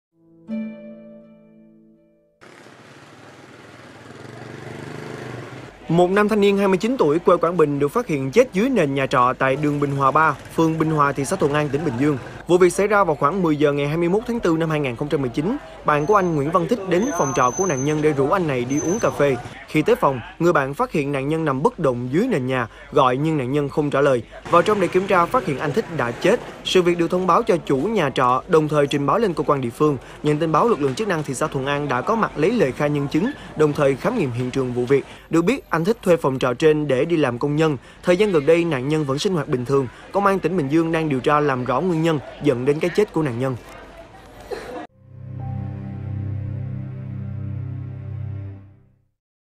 Rủ bạn đi uống cà phê, phát hiện bạn chết dưới nền nhà trọ Thời sự Báo Thanh Niên
Trang tin Bố Trạch Online